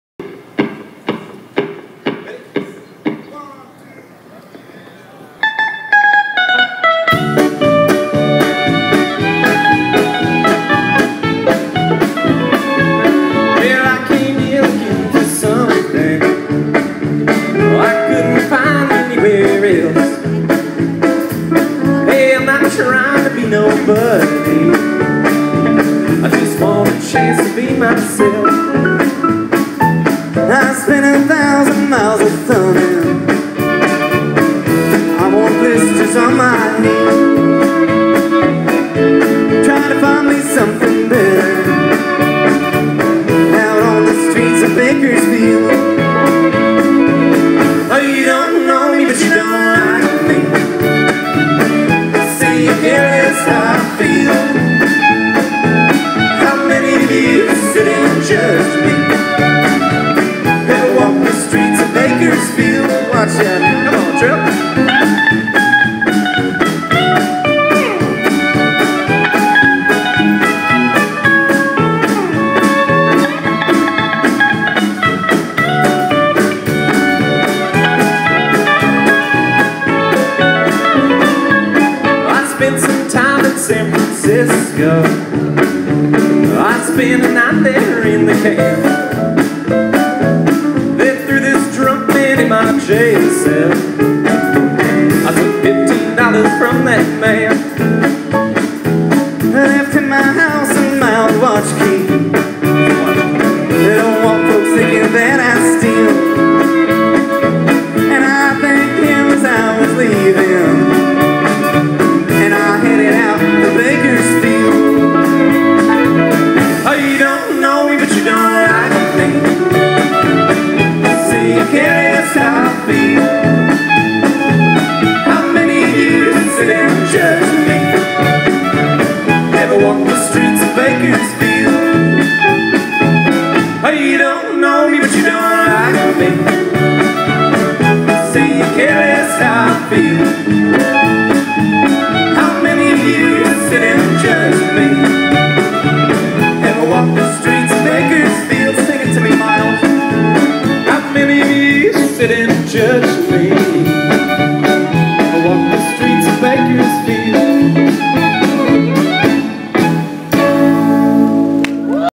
Country Lips featuring Miles Burnett - (Live at the Fremont Fair 02013) [c]
Pucker up! This rebel rockin' band by the name of Country Lips is shown here playing  Seattle Solstice style at the Fremont Fair! See these bands on the Redhook Main Stage, Reyka Vodka Stage and the Fremont Abbey Stage in this video from at the Fremont Fair in Seattle, WA on June 23, 02013.